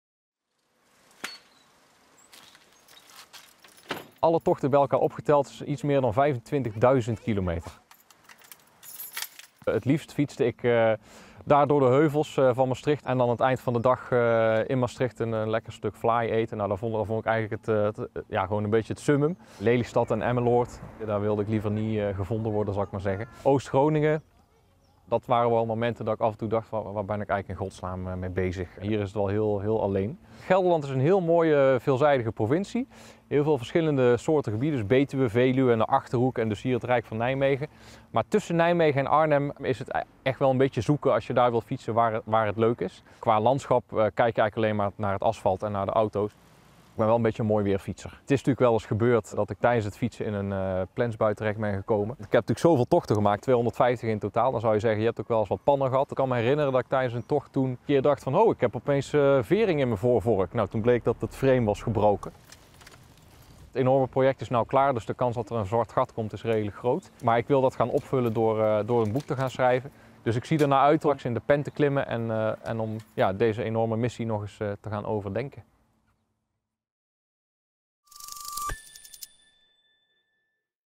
Bram fietste alle plaatsen in Nederland af: 'Gelderland is geweldig'
Nijmegen - Van de uitgestrektheid in Noordoost-Groningen tot de boomgaarden en stadjes in de Betuwe: Bram Witvliet uit Nijmegen zag het allemaal op de fiets. Sinds 2007 fietste hij alle dorpen en steden in Nederland door: "Limburg was het mooist, Drenthe staat op twee, maar Gelderland echt direct daarna op plaats drie."